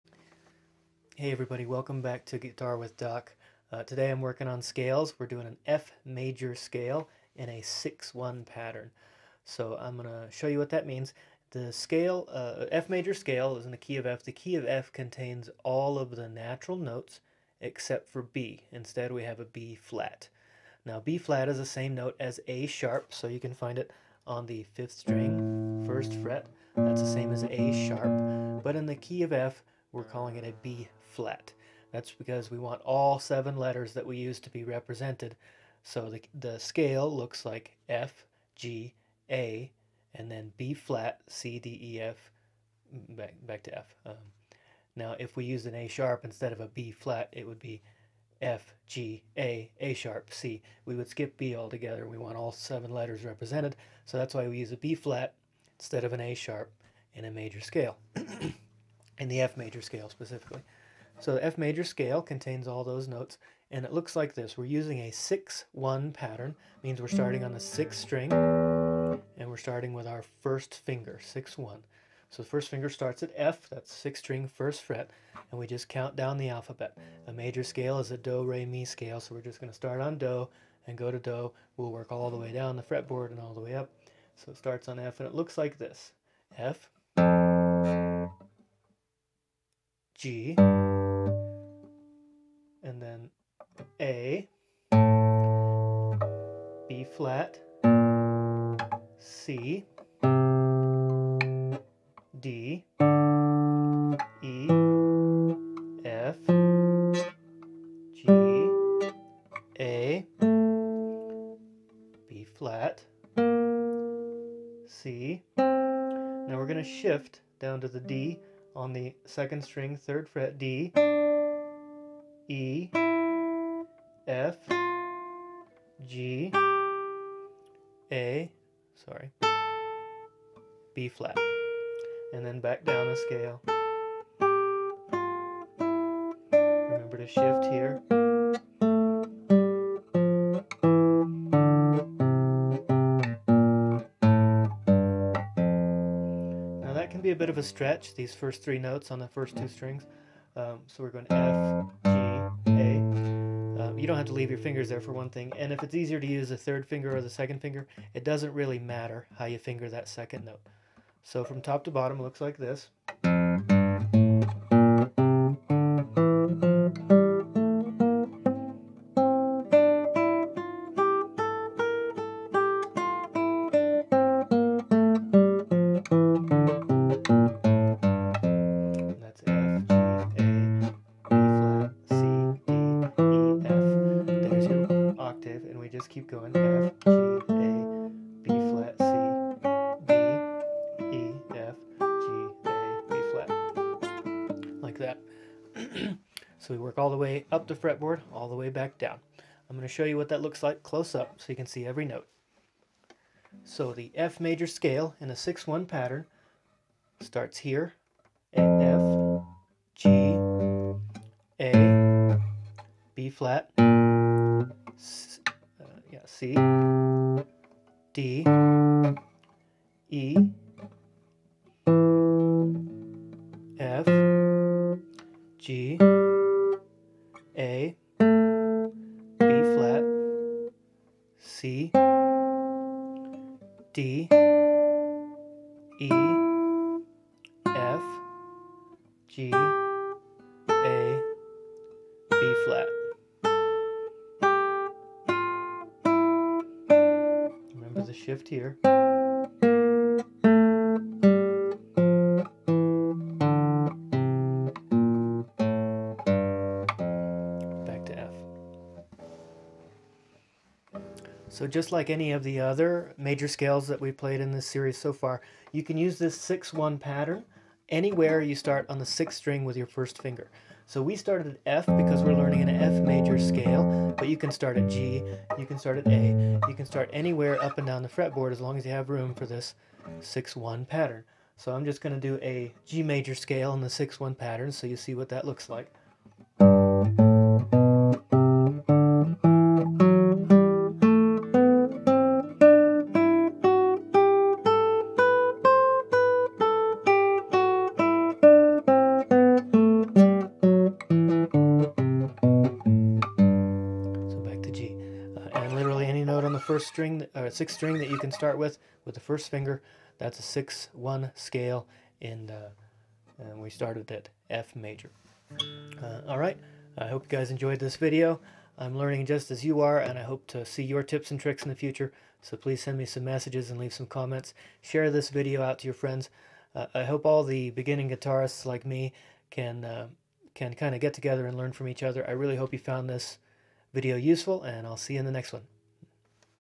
Learning scales for guitar. F Major scale in 6-1 pattern. Major scales part 4 (Guitar with Doc)
F Major is a commonly used scale that is useful to any guitarist. We are going to learn this popular scale while also learning a super useful new scale pattern that you can apply anywhere on the fretboard. We are using the 6-1 (6th string, 1st finger) pattern to do a Do-Re-Mi scale starting at the F on the 6th string, 31st fret.

The F major scale contains the following notes in order from the root: F G A Bb C D E F, with B flat being the only non-natural note.

All Major scales follow the same basic pattern:
Tone, tone, semitone, tone, tone tone, semitone.

Or, if we were to number the notes starting with 1 as the root note like this:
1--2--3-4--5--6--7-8

You can see that there is a semitone between 3 and 4, and between 7 and 8. Hope this helps!